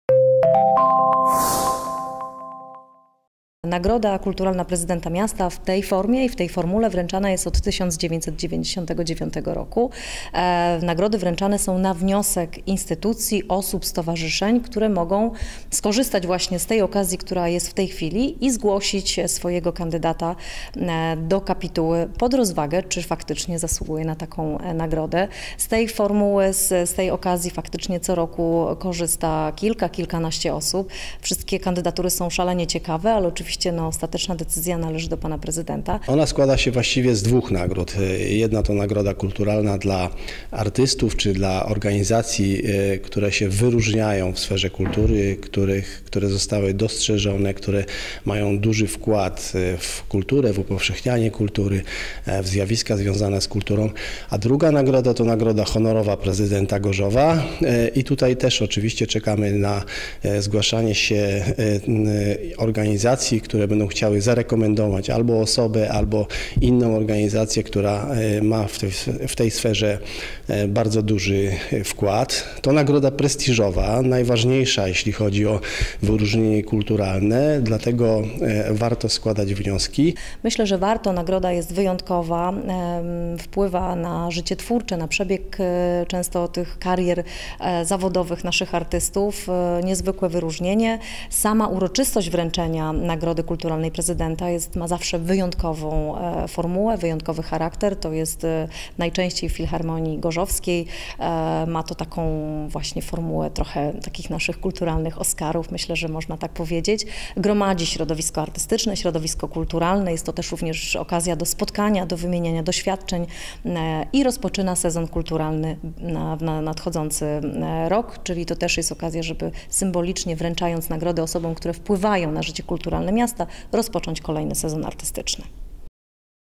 Zaledwie kilka dni pozostało na zgłoszenie wniosku o przyznanie Nagrody Kulturalnej
Tak zwane „Motyle” zostaną wręczone 1 października podczas Inauguracji Sezonu Kulturalnego w CEA Filharmonii Gorzowskiej.